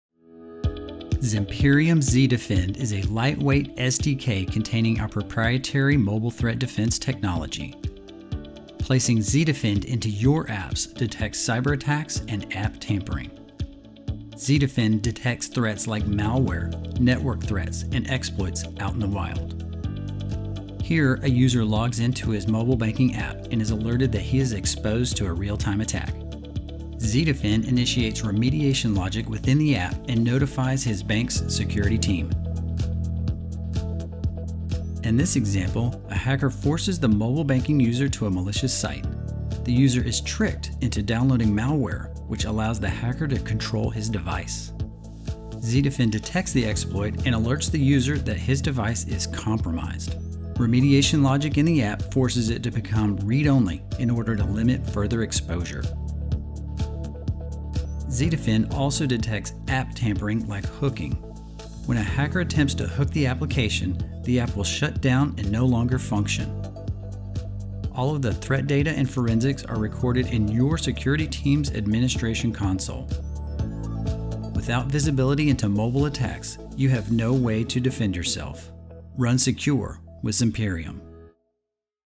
zDefend | How to Embed Mobile App Protection to Detect Cyberattacks
Zimperium zDefend is a lightweight SDK containing Zimperium’s proprietary mobile threat defense technology. Placing zDefend into your apps detects cyber-attacks and app tampering.

zDefend detects threats like mobile malware, network attacks, exploits, and compromised devices. If an app user is attacked, zDefend initiates remediation logic within the application and notifies the application developer’s security team. All of the threat data and forensics are recorded in a central administration console.

Without visibility into mobile attacks, you have no way to defend your application from attacks.